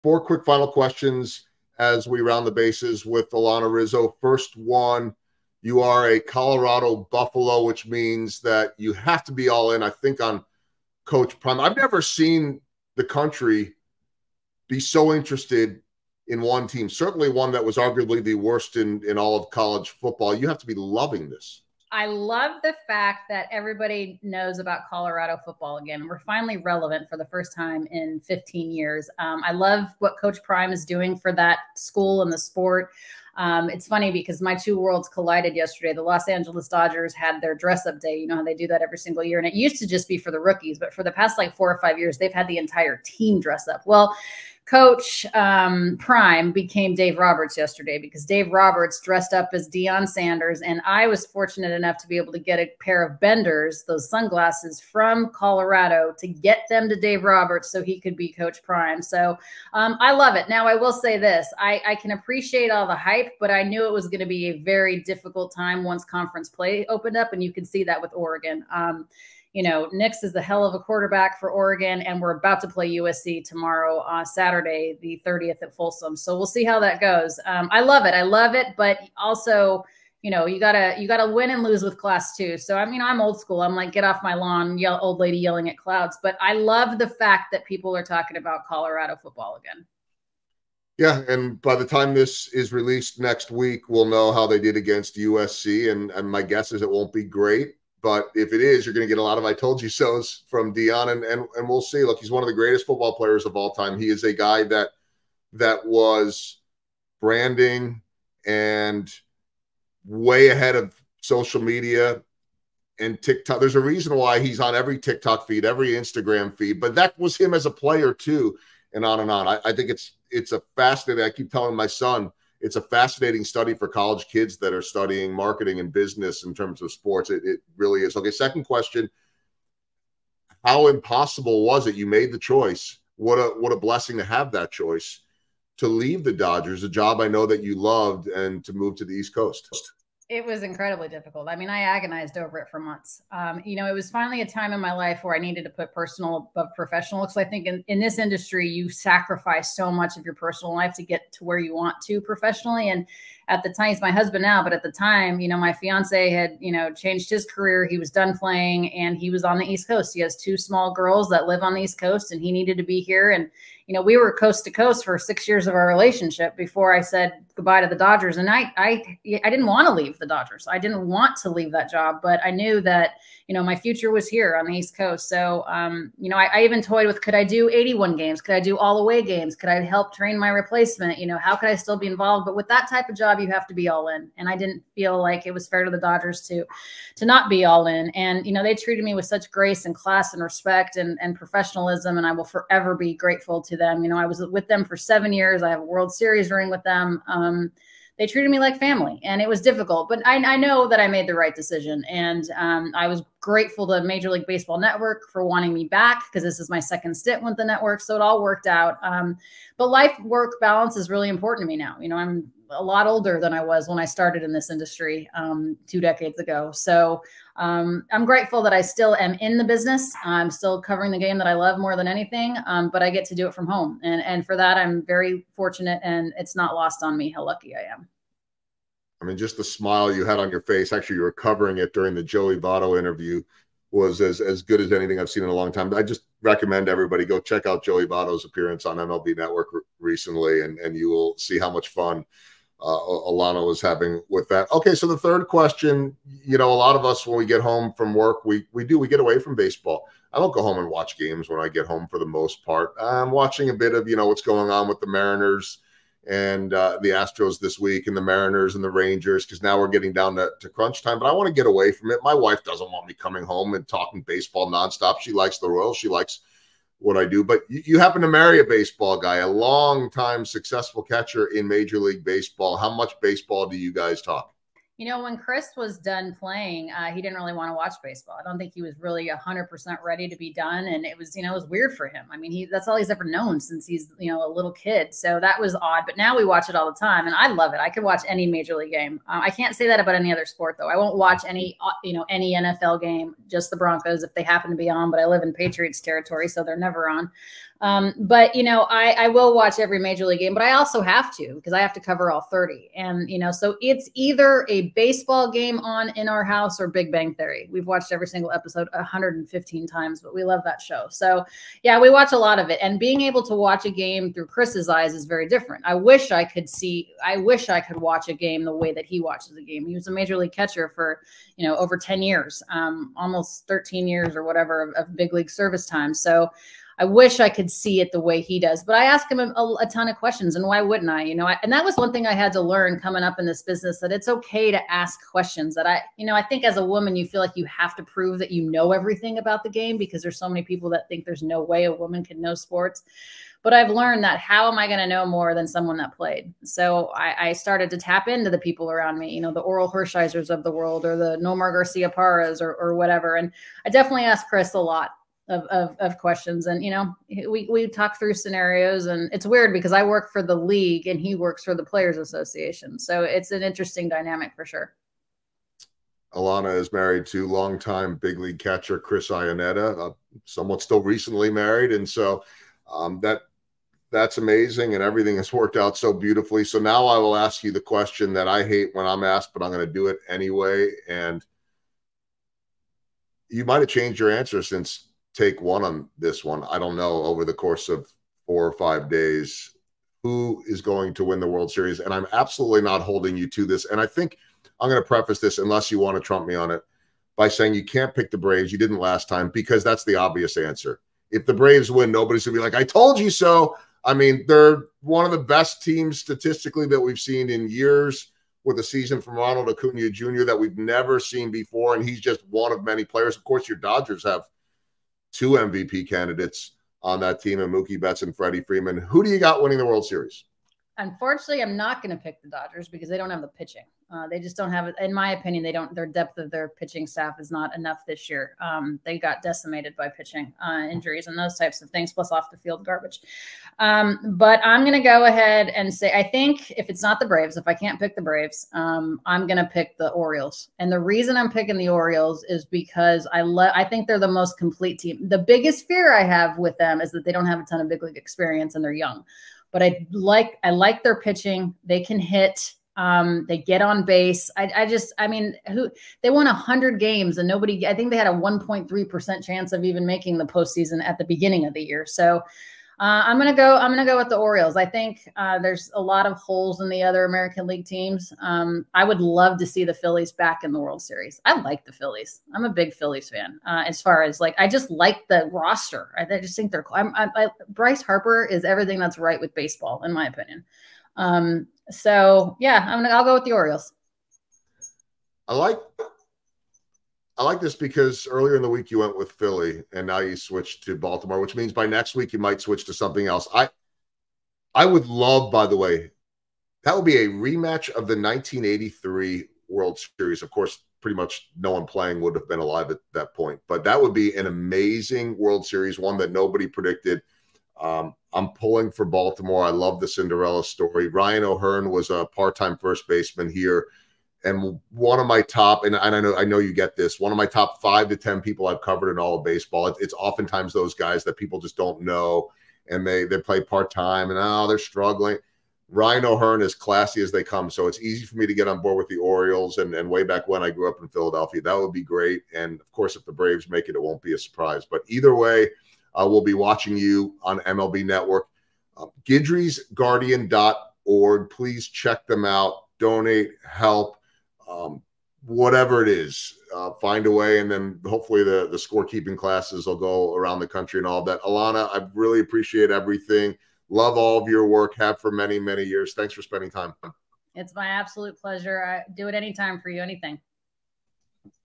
RTB Extra Innings with Alanna Rizzo
Alanna Rizzo is one of many the powerful female role models in sports broadcasting. You can see her on MLB Network, and while she may be best known for the indelible mark she left on Dodgers Nation, her work as a philanthropist is what has changed - even saved - countless lives. Alanna Rizzo is an Emmy Award-winning sportscaster and the President of Guidry’s Guardian, a foundation dedicated to finding every dog a home. As a reporter, her talent has always shined most when telling behind the scenes stories. And her latest one - though bold - just may be her most promising one to date…for ball players - and ball-playing pups - alike.